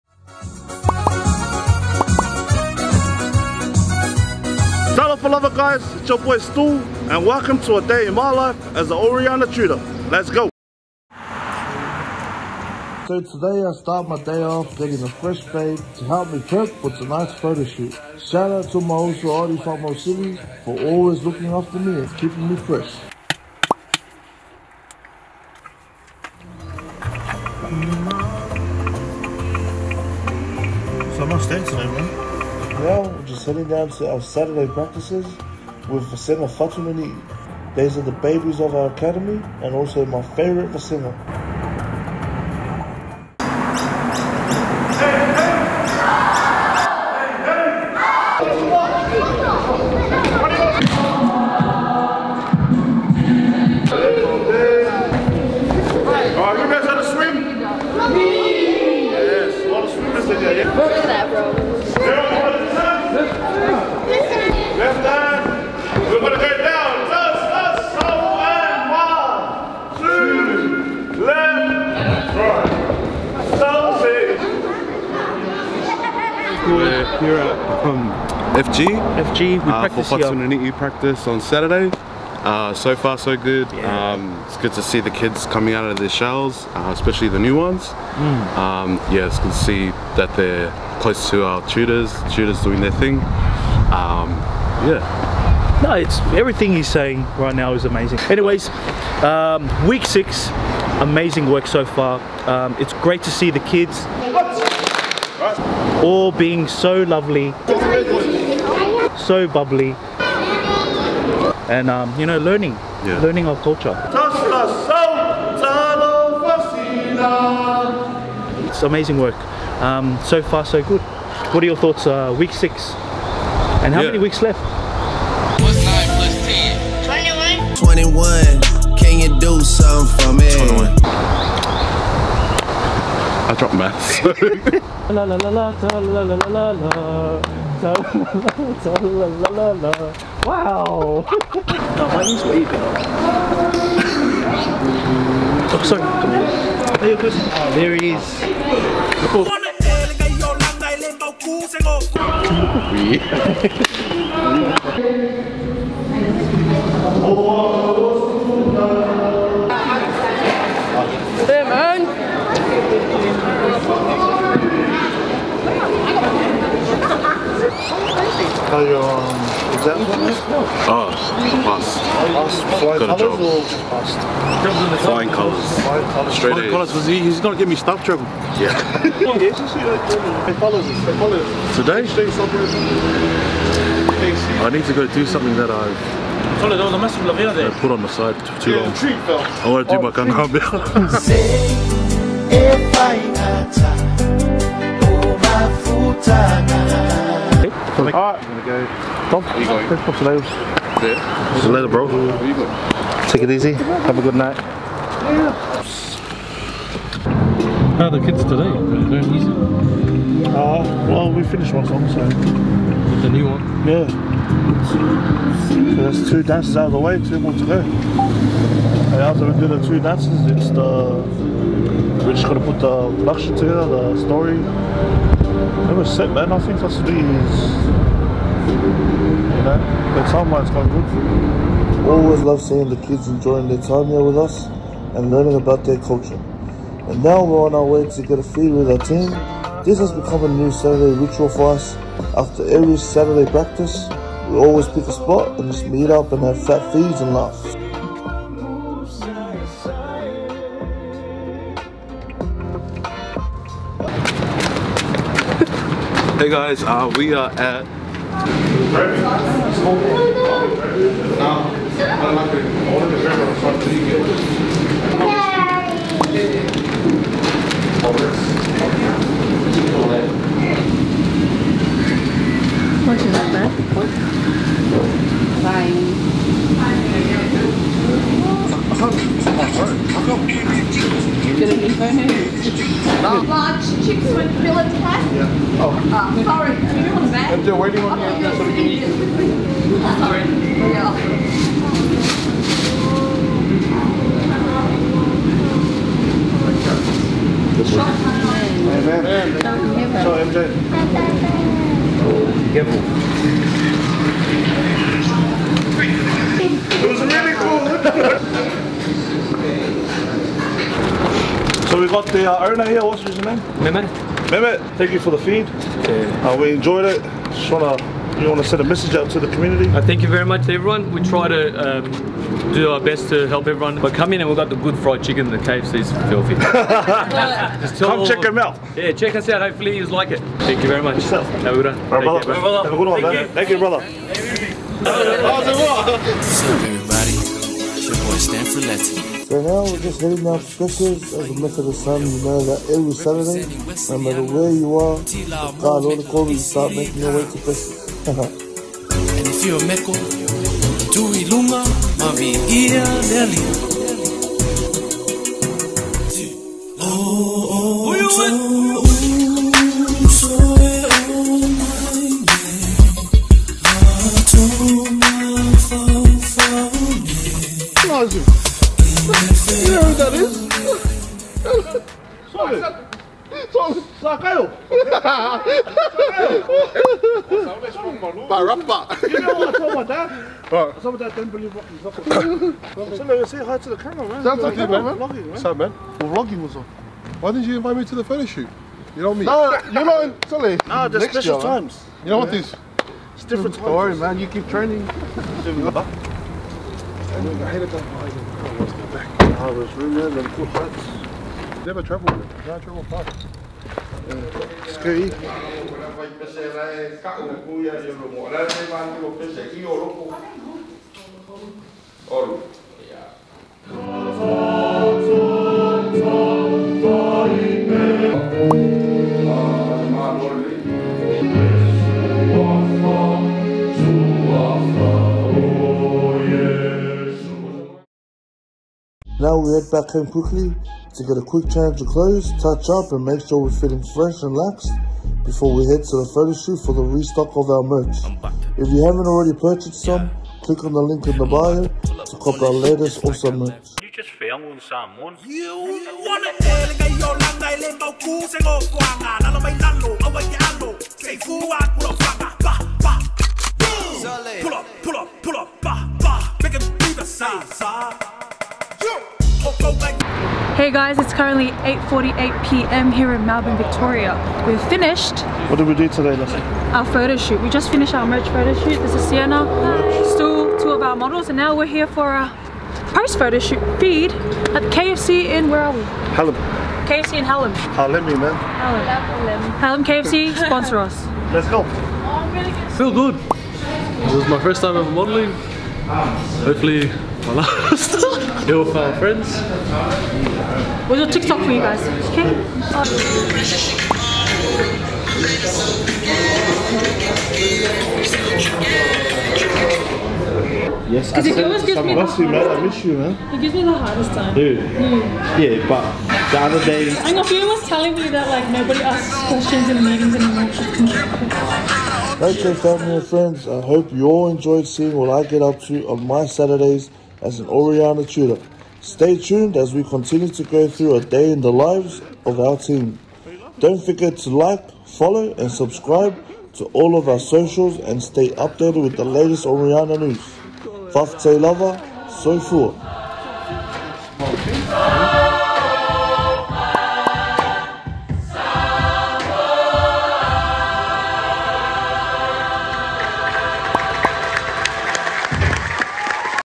A DAY IN OUR LIVES Ep. 1 w/ STU | PART-TIME MODEL!?!?!?
Follow one of our tutors STU on a particularly eventful practice day. See what he gets up to behind the scenes!

Big thank you to Odi Fa'amausili @ Forza barbers for keeping ya boy fresh!

And a big thank you always to our friends and family for continuing to support us.

Fa'afetai lava,
OSA.